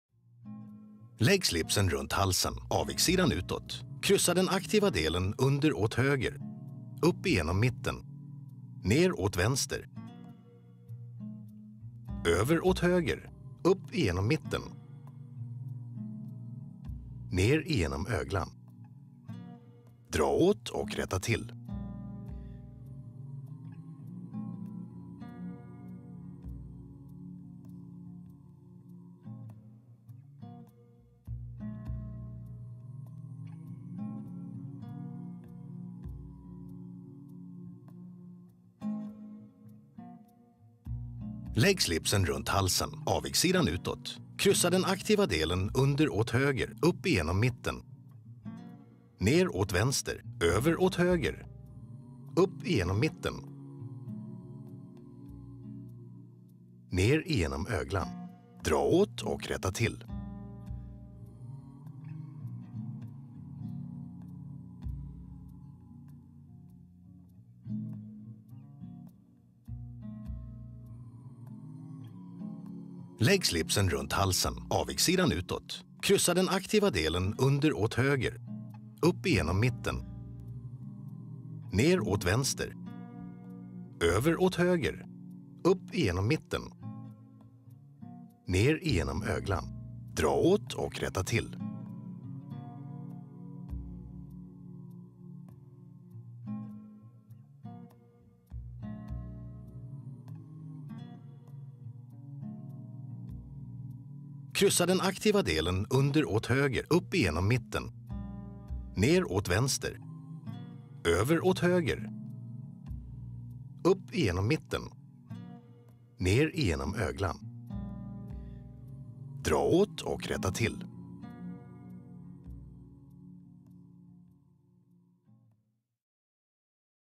Nicky knut | Knyta slips
Learn the Nicky knot step by step; a great button for skinny ties. Filmed from four different points of view, success is guaranteed.